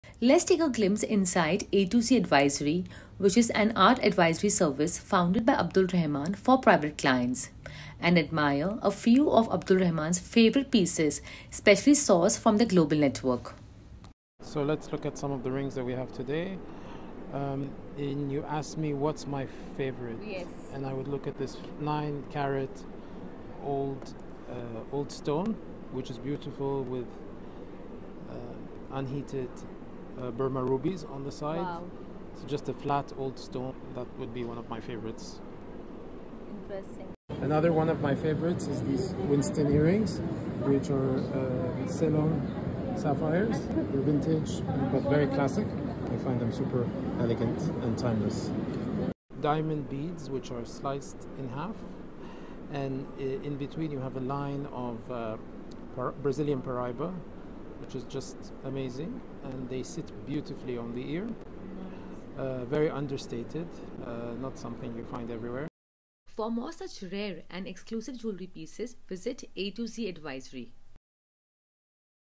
Renu meets A2ZAdvisory
RenuRecommends A2Z @a2zadvisory at @jewelleryarabiabahrain
The Art Advisory Service!
Let’s take a glimpse inside the A2Z Advisory, an art advisory service founded by Abdulrahman H. Al-Zayani for private clients and admire a few of Abdulrahman’s favourite pieces, specially sourced from their global network!
Let me know which one appeals to you the most!